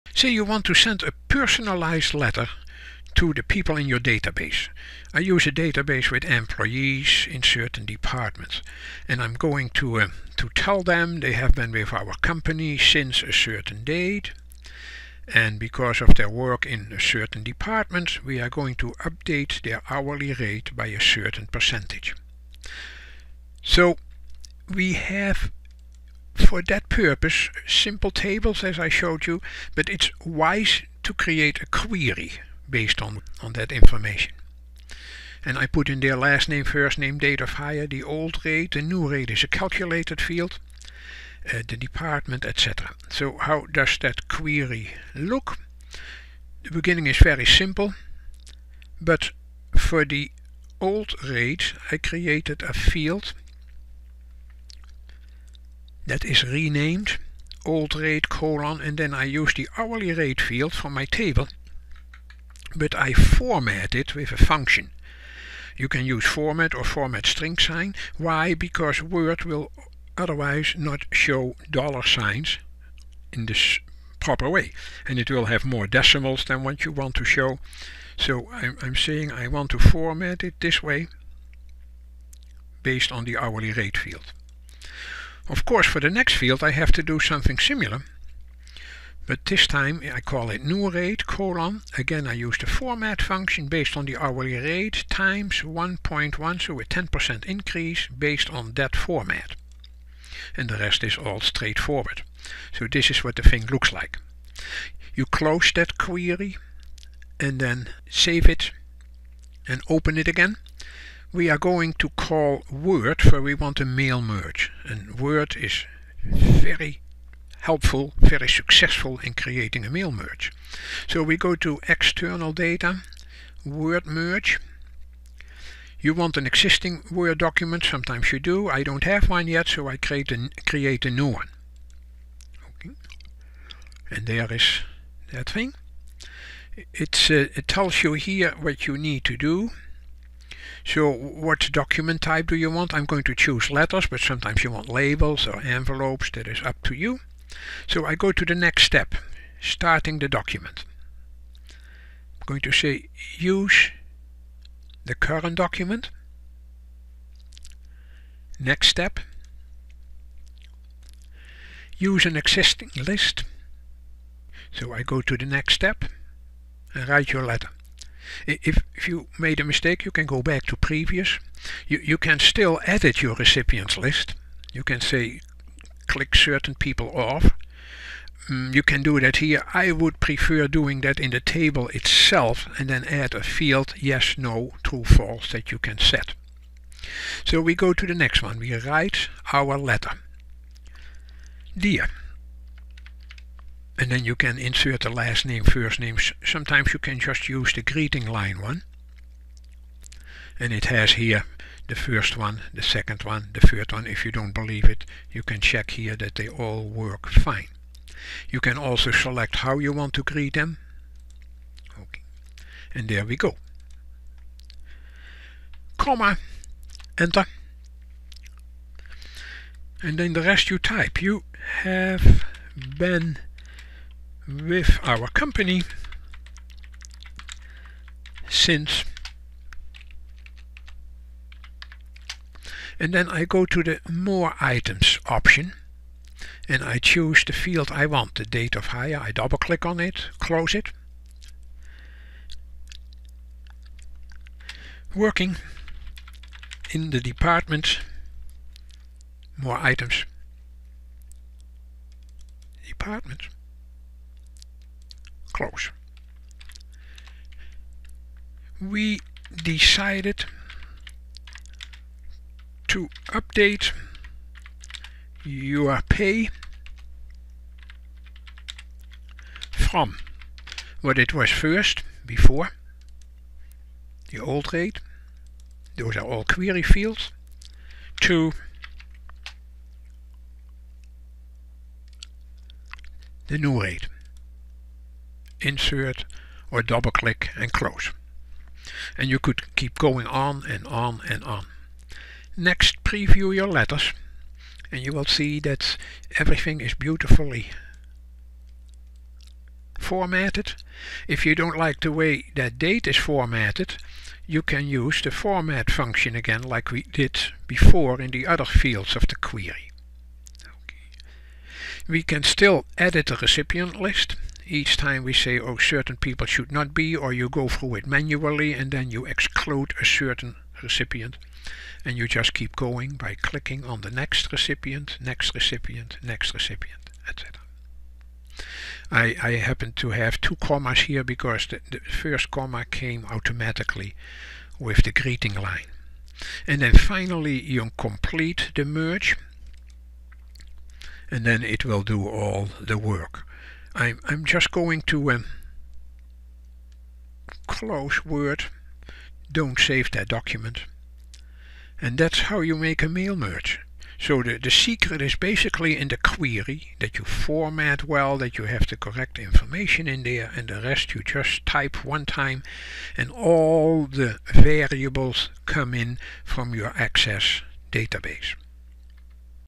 Mail Merge from an Access Database
To create letters, envelopes, or labels for people, companies, or organizations stored in your database, you need some simple tools. Here they are...